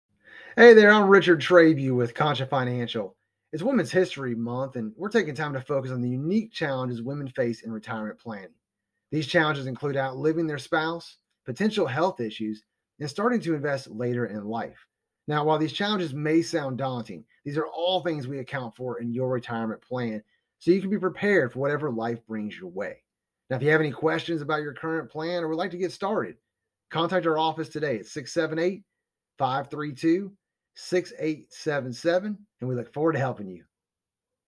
Unique Challenges and Solutions for Women in Retirement
March is Women’s History Month, and women continue to play an increasingly important role in finance. 44 of the companies on the 2022 Fortune 500 list were led by females.1  As we celebrate the incredible accomplishments of women, we have to acknowledge the potential risk factors they face when it comes to retirement planning. Let’s dive into some of the potential risk factors below. Women Are More Likely to Outlive Their Spouses If you’re a 65-year-old woman, statistics say you have another 20.7 years of life left, compared with 18.1 years for the average male.2 To add to the fact that women will need income to live off of for years after their spouse has passed, this stat also affects Social Security benefits. For women who were stay-at-home caregivers, didn’t work out of the home, and therefore do not receive Social Security benefits themselves. More Likely to Experience Health Issues Women might live longer than their male counterparts, but they also experience more health issues. Among the people who require nursing home care, 70 percent are women. And, for women who need long-term care, they’ll need it for 3.7 years (on average). With the average monthly cost of a private nursing home at $8,910, that adds up to $395,604, which can quickly eat into retirement savings. Start Investing Later and Are Less Risk Averse Only 14 percent of women frequently discuss saving, investing, and planning for retirement with family and friends.3 While it’s a common idea that women invest in less risky investments, it may also come back to the wage gap. With less income, women may be less willing to invest that income because it’s deemed as necessary for daily living. Here are some things women can do to face these challenges: • Understand what’s in your retirement accounts. • Consider a long-term care plan for you and your spouse. • Adopt a healthy lifestyle. • Save regularly. • Embrace lifelong learning. • Talk to a financial professional about establishing a written retirement income plan.  Our goal is to help you retire with added confidence. We look forward to helping you!